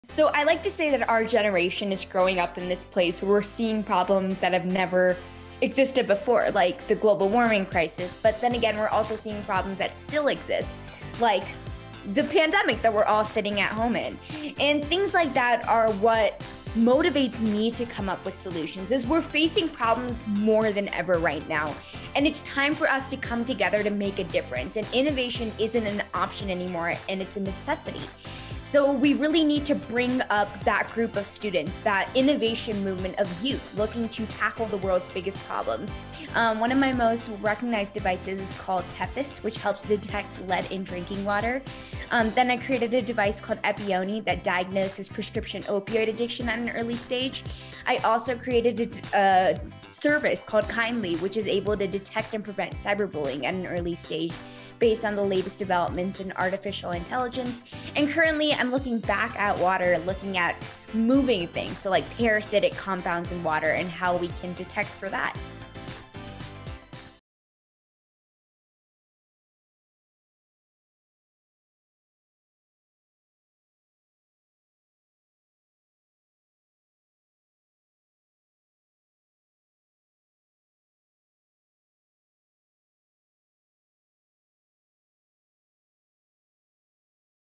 This is Time's Kid of the Year: Gitanjalia Rao
Time Magazine’s Kid of the Year, Gitanjalia Rao, has invented devices and solutions for opioid addiction, cyberbullying and water pollution.

She’s only 15 years old.